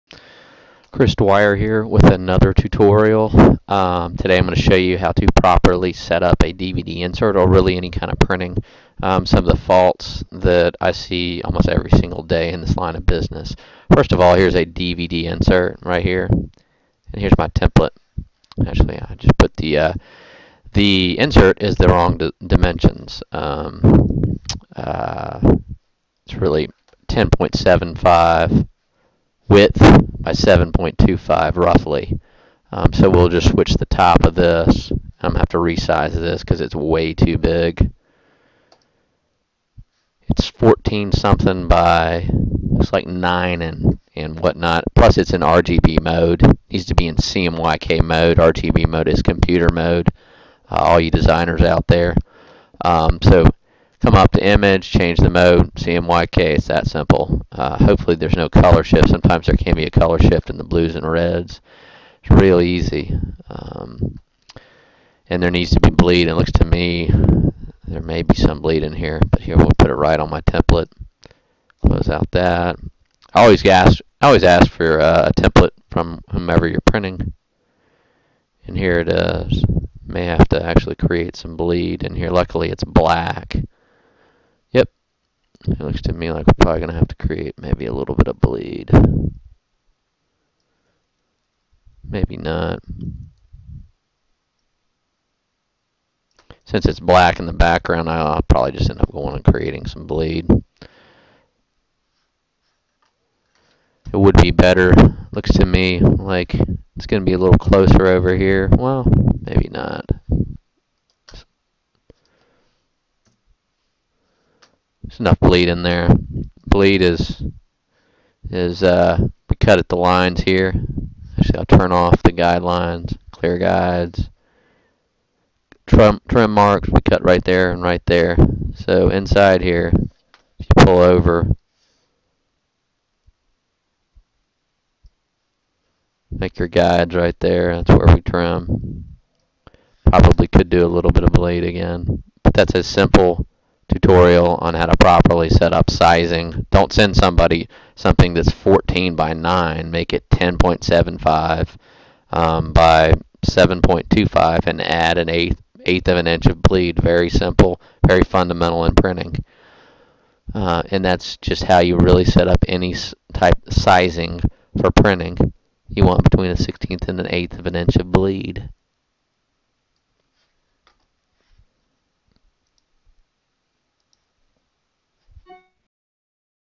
File Setup for printing Creating Bleed
File Setup for printing:
1. Cutting in printing needs bleed.
2. Make sure to save in CMYK not RGB mode. CMYK is print mode.